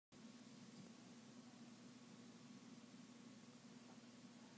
Lego minifigure series 3 (#4 on checklist)
This is the samurai warrior from series 3. He comes with armor and a katana. You can find him at #4 on the series 3 checklist.